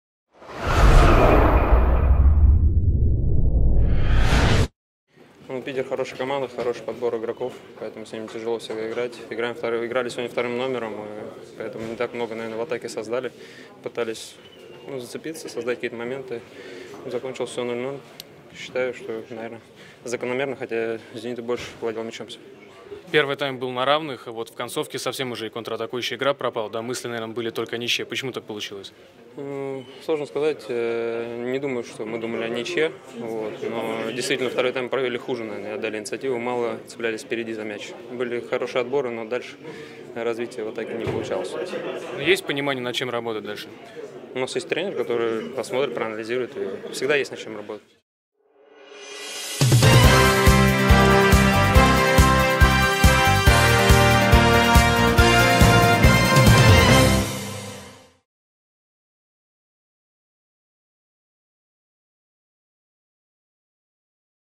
Тарасов: Второй тайм провели хуже первого
Полузащитник «Локомотива» Дмитрий Тарасов резюмировал нулевую ничью с «Зенитом» в первом туре РОСГОССТРАХ Чемпионата России.